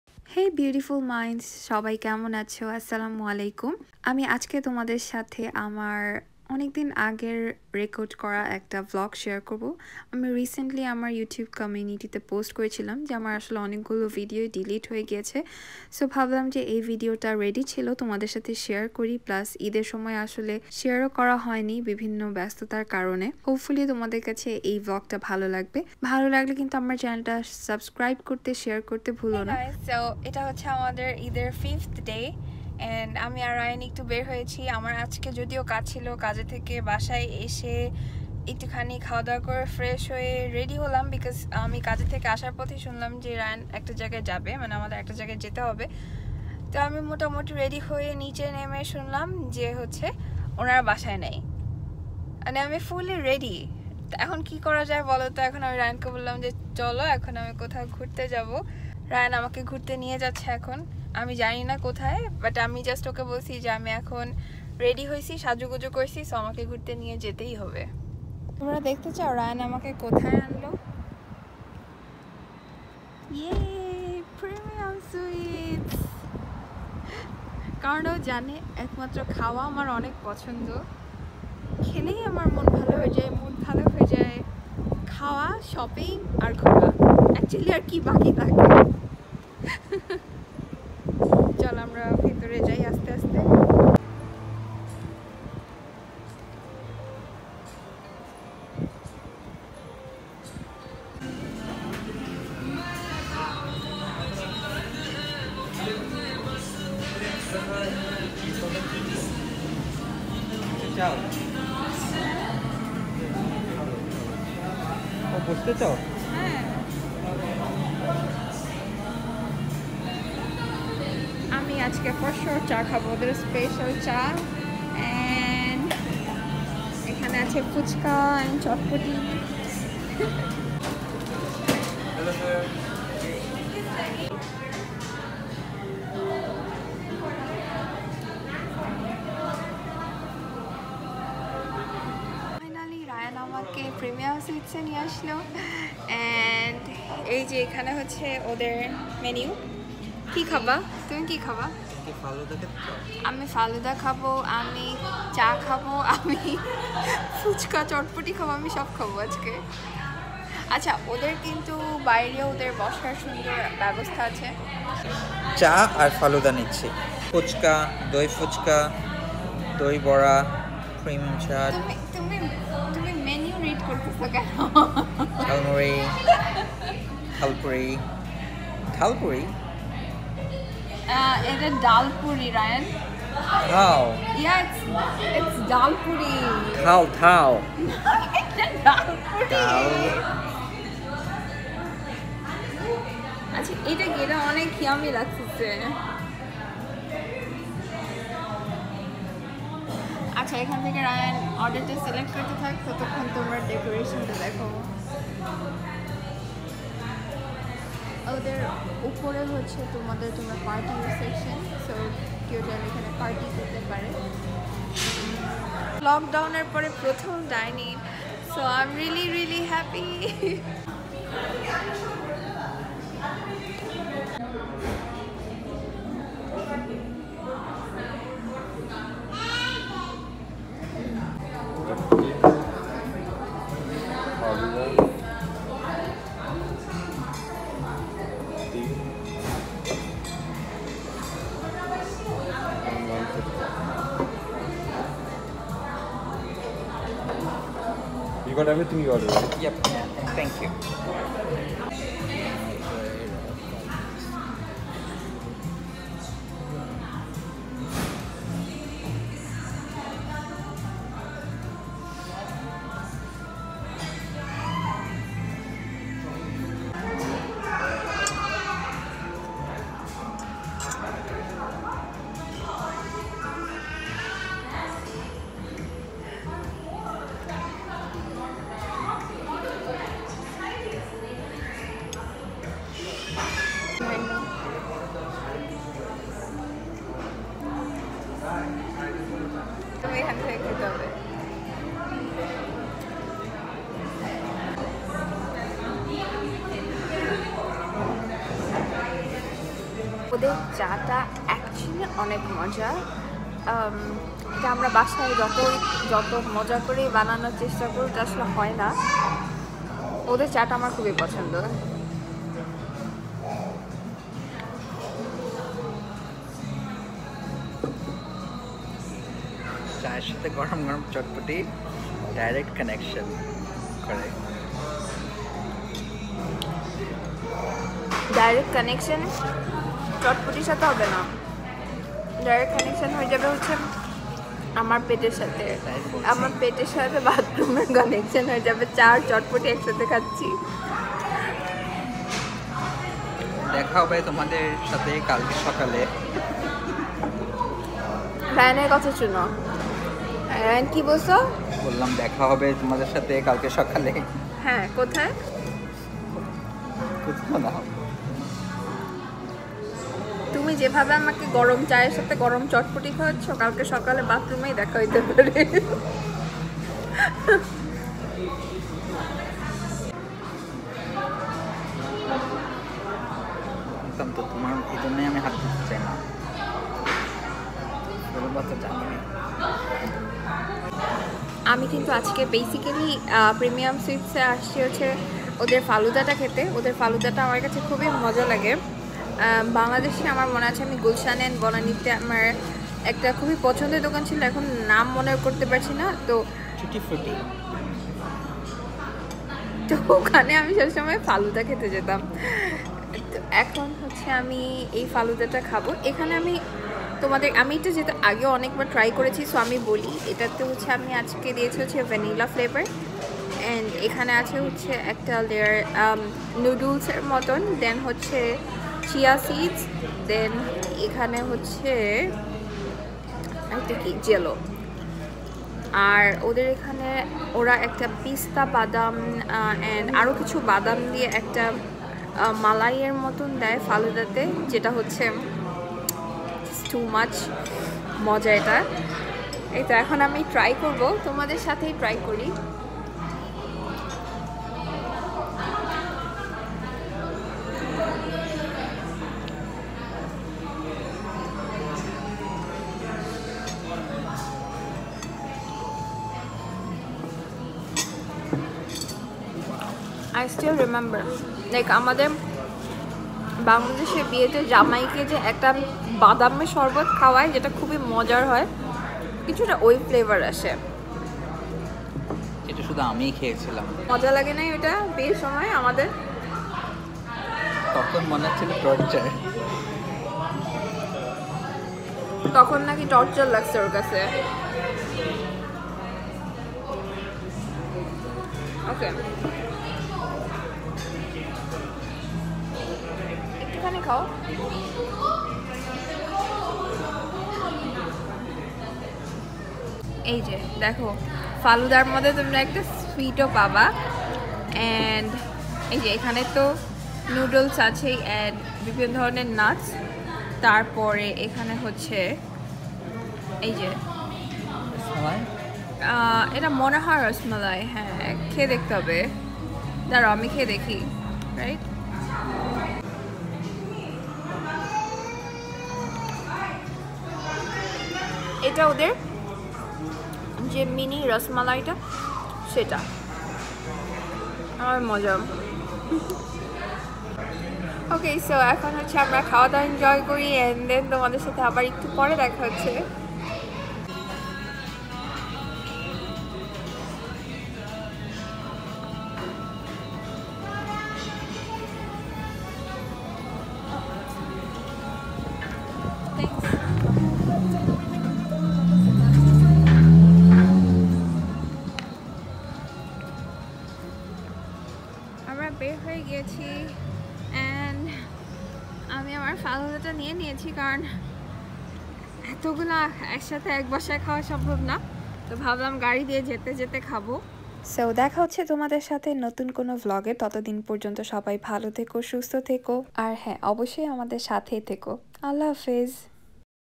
Qurbani Eid e Tona tunir ghuraghuri | Eid day 5 | 2021 | Premium Sweets
Assalamualikum.
Sobai kemon acho ? Ei bochor qurbani eid er 5th day te amara ektu ghuraghuri r khawadawa korechi . Sei chotto vlog tay tomader sathe share korlam .